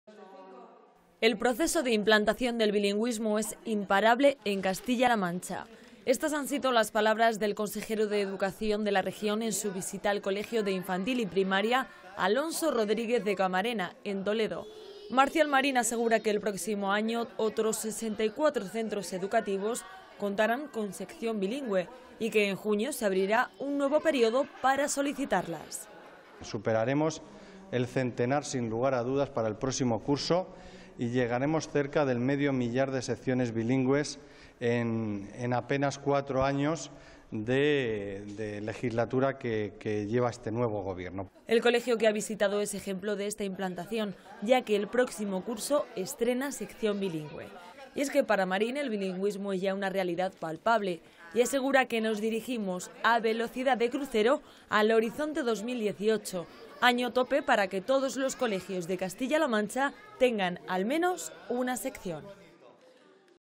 Marín: "El bilingüismo ya es una realidad en nuestra región"
En 2015 cerca de 500 Centros Escolares de Castilla-La Mancha contarán con una sección bilingüe. De este modo nuestra comunidad se acerca al objetivo marcado para 2018, año en el que, según las previsiones del gobierno, todos los centros tendrán una.